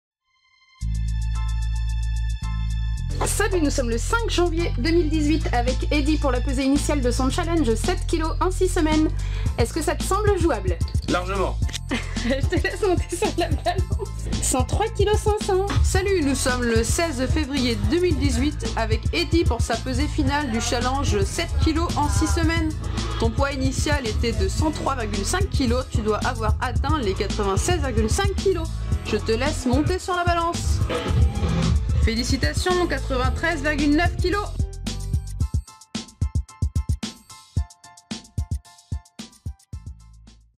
Transformation physique rapide Angers
Eddy a perdu 9,600 kgs en 6 semaines à la Training Academy